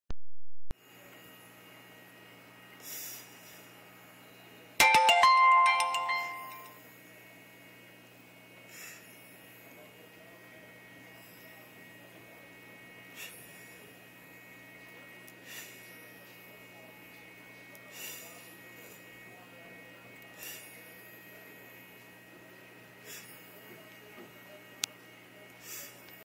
Silver Tip Pulsing Xenia
Silver Tip Xenia in moderate flow. This will pulse w/o flow. 28 Gallon Nano Cube. 450 PPM Ca levels. Scooter Blenny is a ham and gets in all my vids!